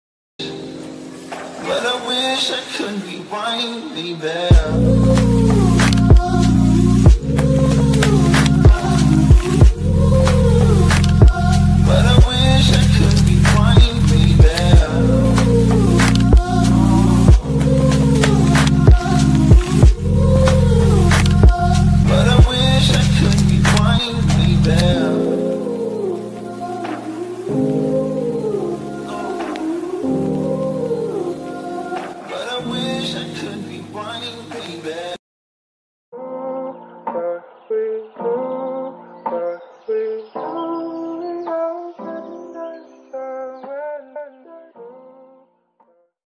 Rewind Meme//FlipaClip
Oh hi , this is a positive stress relief
It's supposed to reflect that sometimes I can get upset but theirs always something/someone that can make me feel better.
So I finished this all last night

Original -

Song-

Everyone asks what I use even though it says it here and in the title ;-;

Flipaclip
Kinemaster
My finger